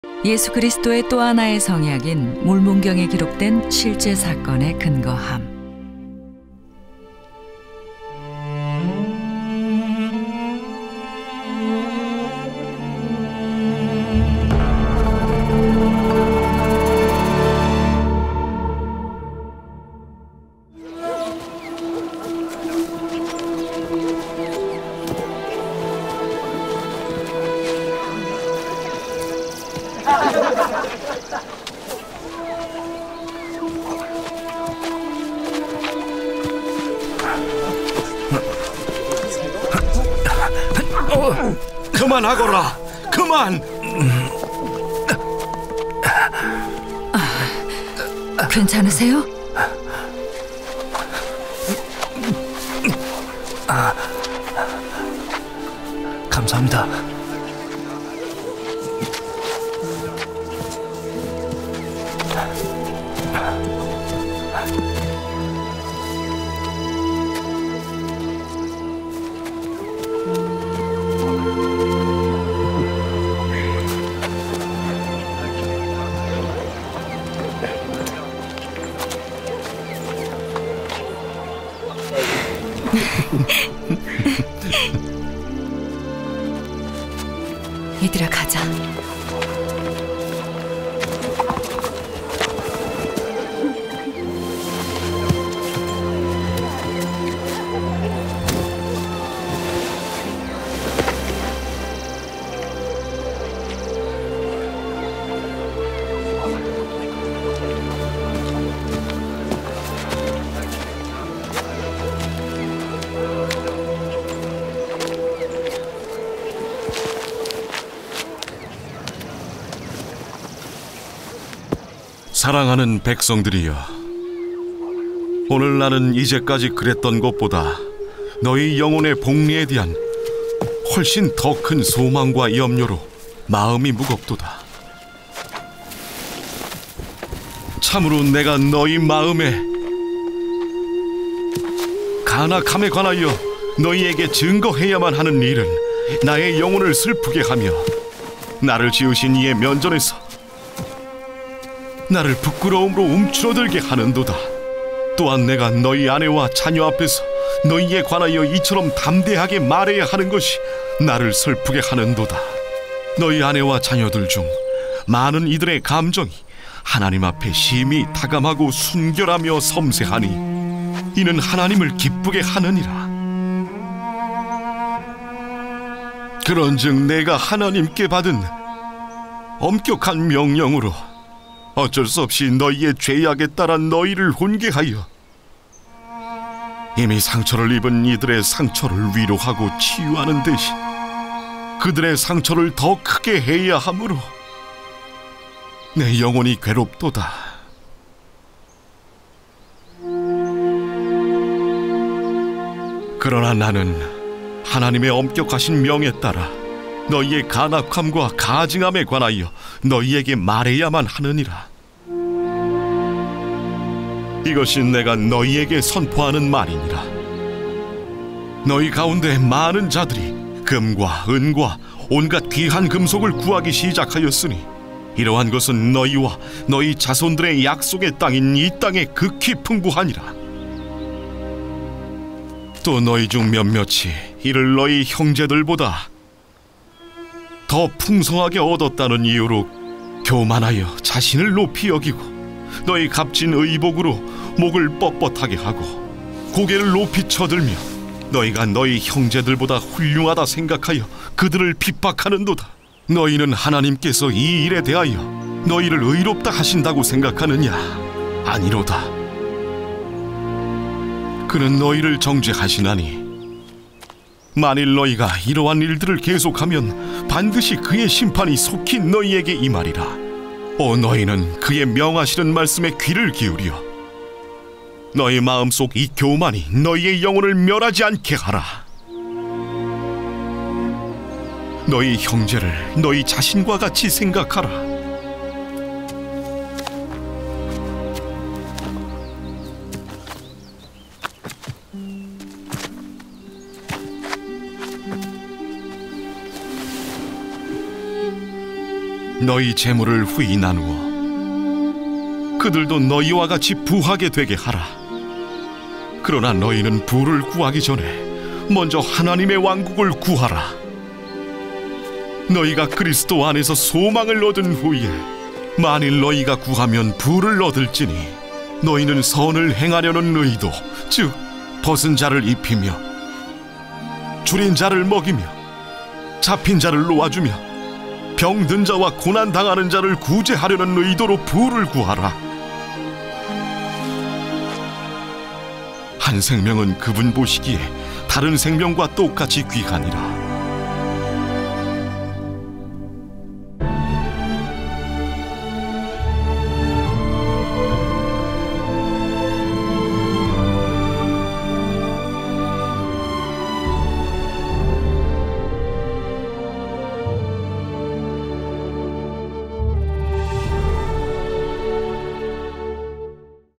야곱이 교만에 관해 가르치다 | 야곱서 2:3~19
몰몬경의 선지자 야곱은 자신의 백성에게 교만하지 말고 형제들을 핍박하지 말라고 경고한다. 그는 백성들에게 하나님의 왕국을 먼저 구하라고 가르친다.

야곱서 2:3, 6~7, 10, 12~13, 16~19:
3 또 내가 지금까지 나의 부름 받은 직분을 부지런히 수행하여 온 줄을 너희 스스로가 알거니와, 나는 오늘 이제까지 그랬던 것보다 너희 영혼의 복리에 대한 훨씬 더 많은 소망과 염려로 짓눌리고 있느니라.
6 참으로 내가 너희 마음의 간악함에 관하여 너희에게 증거해야만 하는 일은 나의 영혼을 슬프게 하며, 나로 하여금 나를 지으신 이의 면전에서 부끄러움으로 움츠러들게 하는도다.
7 또한 내가 너희의 아내들과 너희의 자녀들 앞에서 너희에 관하여 이토록 많은 담대함을 사용하여 말해야만 하는 일은 나를 슬프게 하는도다. 그들 중 많은 이들의 감정이 하나님 앞에 심히 다감하고 순결하며 섬세하니, 이는 하나님을 기쁘게 하는 것이라.
10 그러나 과제의 막중함에도 불구하고, 내가 하나님의 엄격하신 명대로 행하여, 마음이 청결한 자들과 상한 마음을 지닌 자들의 면전에서 전능하신 하나님의 꿰뚫어 보시는 눈길 아래서, 너희의 간악함과 가증함에 관하여 너희에게 고해야만 하느니라.
12 이제 보라, 나의 형제들아, 이것이 내가 너희에게 선포하는 말이니라. 곧 너희 가운데 많은 자들이 금과 은과 온갖 귀한 금속을 구하기 시작하였나니, 이러한 것은 너희와 너희 자손들에게 약속의 땅인, 이 땅에 극히 풍부하니라.
13 또 은총의 손길이 너희 위에 극히 기쁜 미소를 지은지라, 너희가 많은 부를 얻었으되, 너희 중 몇몇이 너희 형제들의 것보다 더 풍성하게 얻었음으로 인하여, 너희가 너희 마음의 교만 중에 자고하여지며, 너희 의복의 값짐으로 인하여 목을 뻣뻣하게 하고 고개를 높이 쳐들며, 너희가 너희 형제들보다 낫다 생각하여 그들을 핍박 하는도다.
16 오 그가 너희를 이 죄악과 가증함에서 벗어나게 해 주시면 좋겠도다. 그리고 오 너희가 그의 명하시는 말씀에 귀를 기울여, 이 너희 마음의 교만이 너희의 영혼을 멸하지 않게 하면 좋겠도다!
17 너희 형제를 너희 자신과 같이 생각하고, 모든 이와 친숙하며, 너희 재물을 후히 나누어 그들로 너희와 같이 부하게 하라.
18 그러나 너희가 부를 구하기에 앞서 너희는 하나님의 왕국을 구하라.
19 그리고 너희가 그리스도 안에서 소망을 얻은 후에, 만일 너희가 구하면 부를 얻을지니, 너희는 선을 행하려는—벗은 자를 입히며, 주린 자를 먹이며, 잡힌 자를 놓아 주며, 병든 자와 고난 당하는 자들을 구제하려는 의도로 그것을 구하리라.